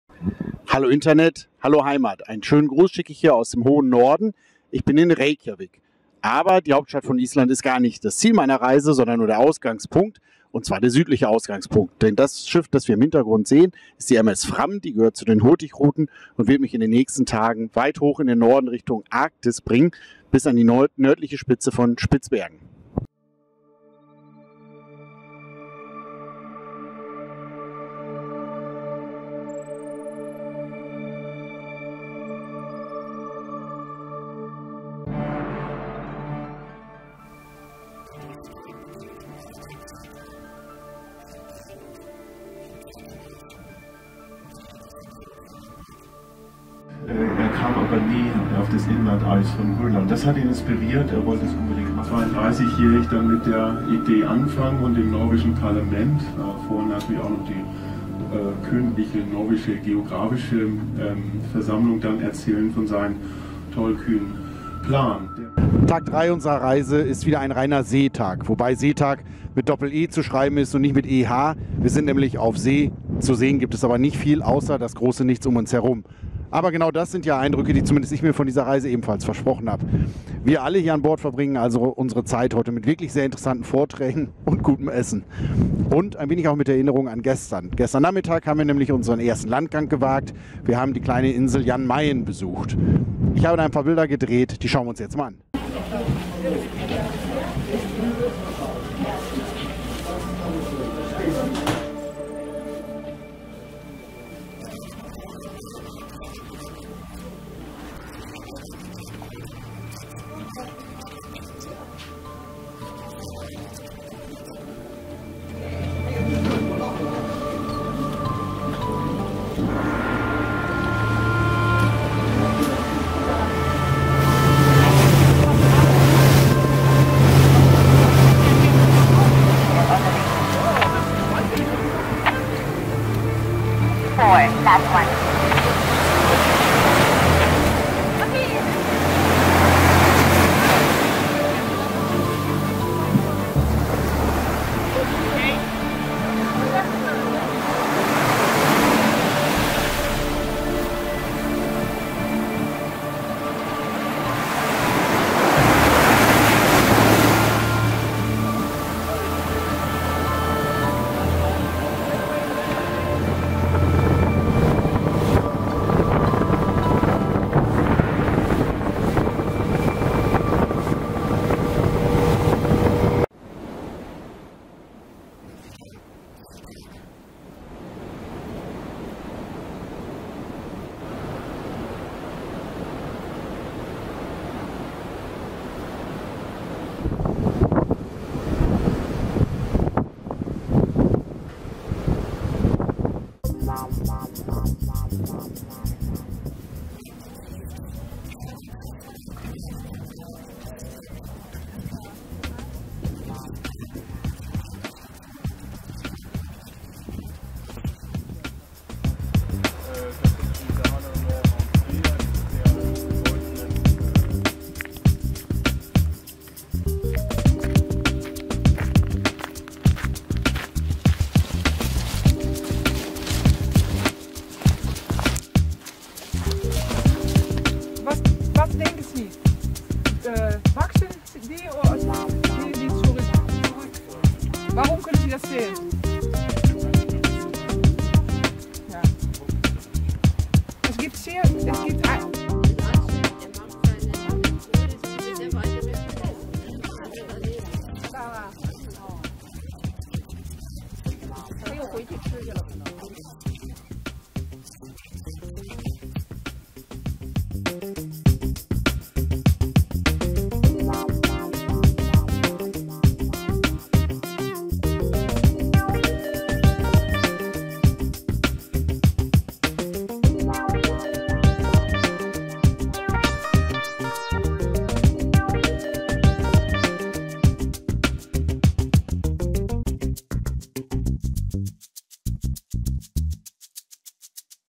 Mit dem Hurtigruten-Schiff MS Fram nach Spitzbergen / Svalbard
"Klimareise": Im Juli 2012 fuhr die MS Fram der Hurtigruten von Reykjavik nach Spitzbergen. Eine Reportage . . .